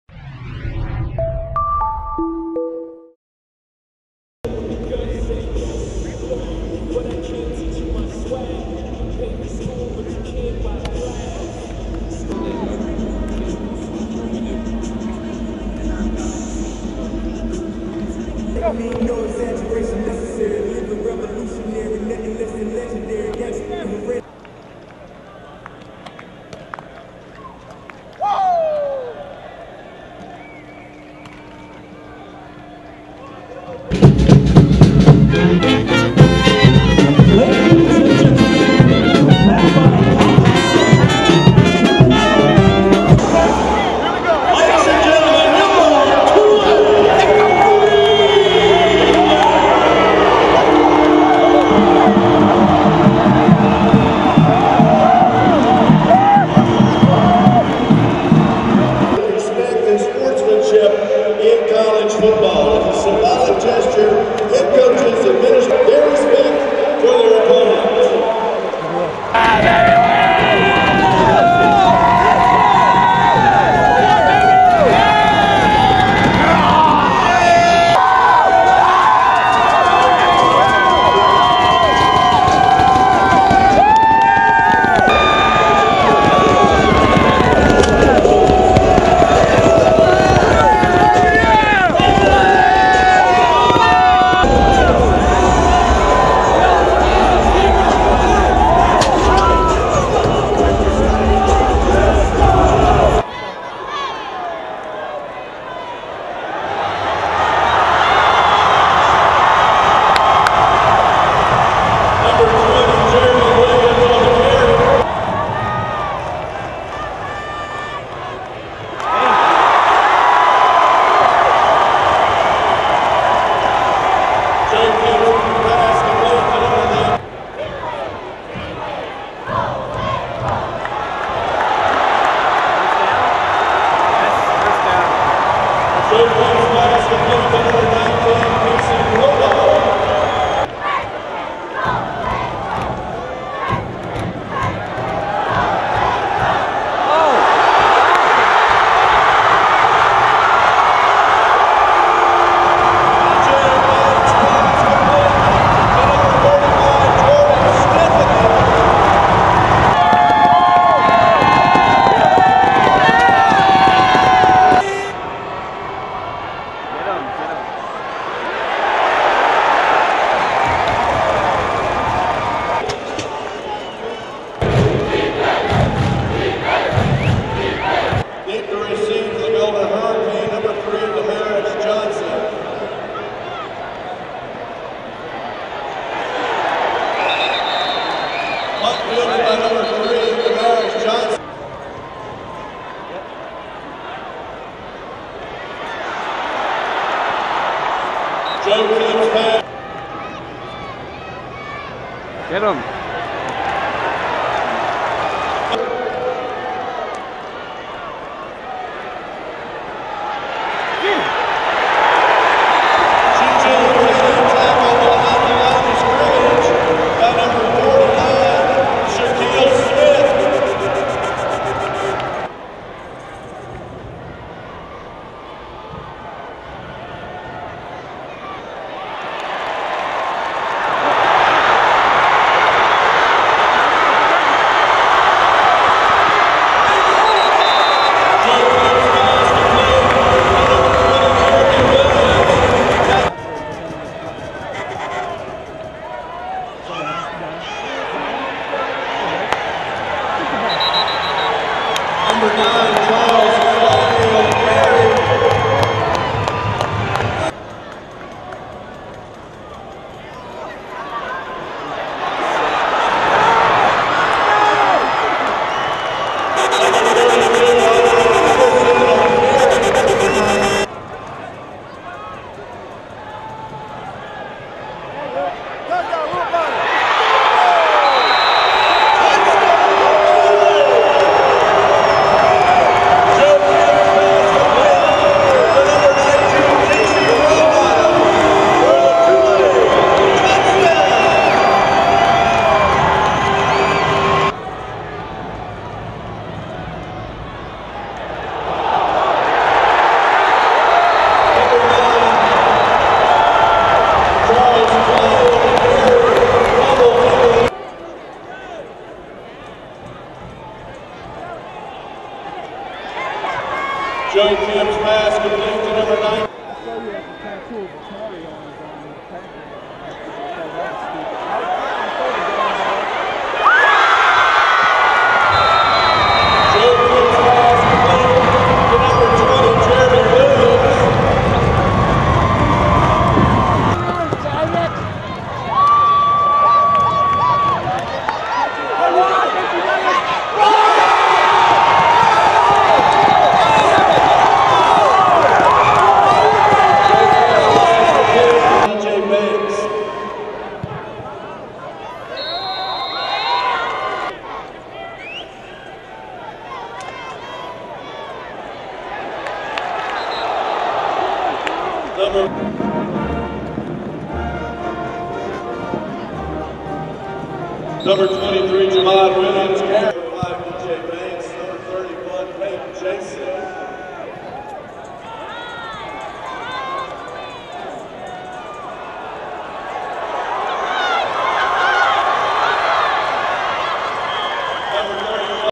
Tulane Football vs. Tulsa 9-4-09 Highlights
Tulane Football Drops Season Opener To C-USA Foe Tulsa, 37-13.

Kemp throws for career-high 278 yards, Robottom snares six passes in the loss.